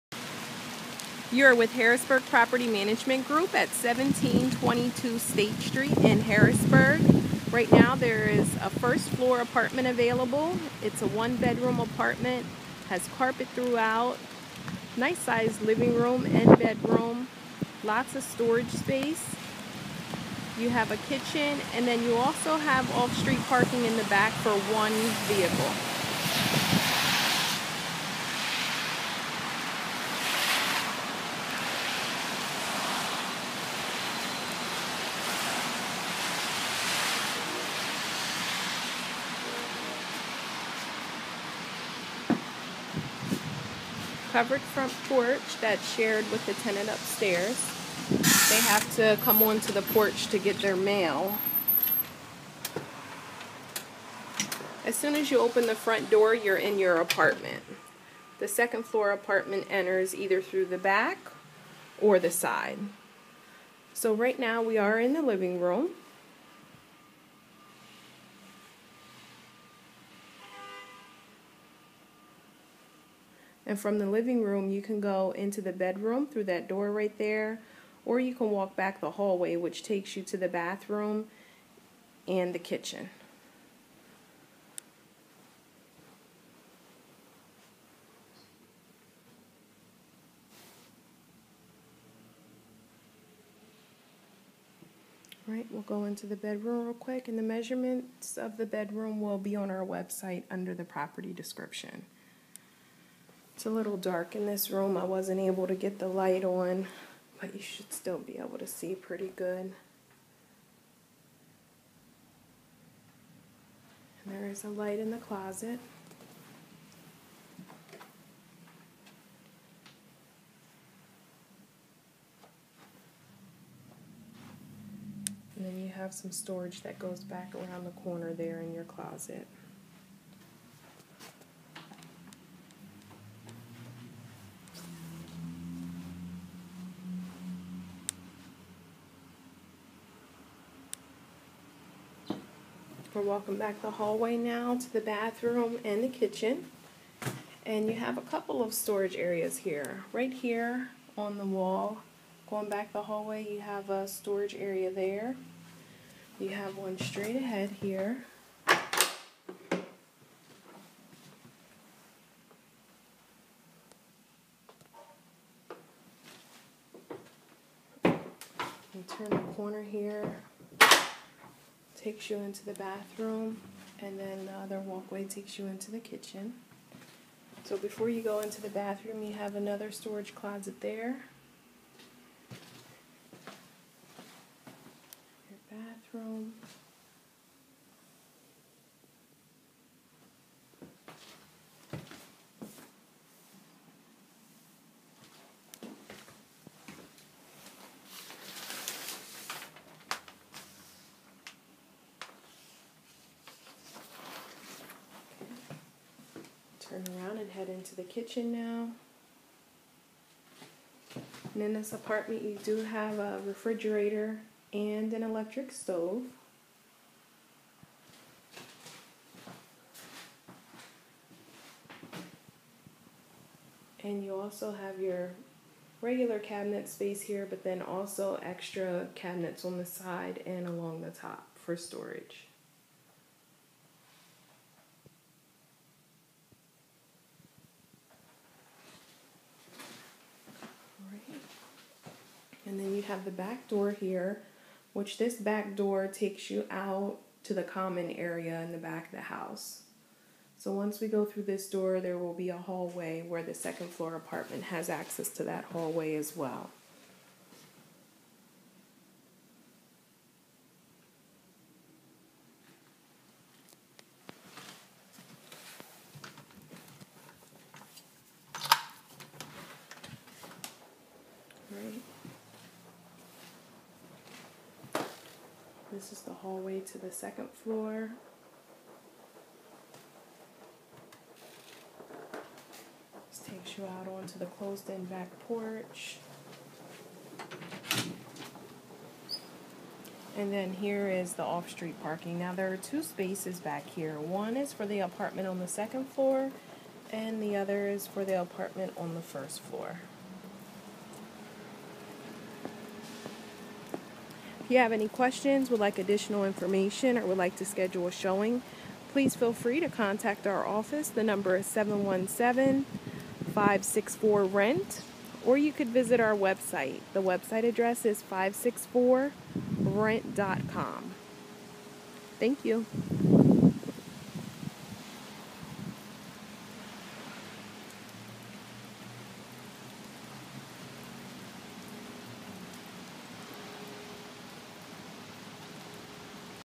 1722 State St, Apt 1, Harrisburg For Rent 11.19.15
1st floor, one bedroom apartment w/ carpet throughout and off street parking.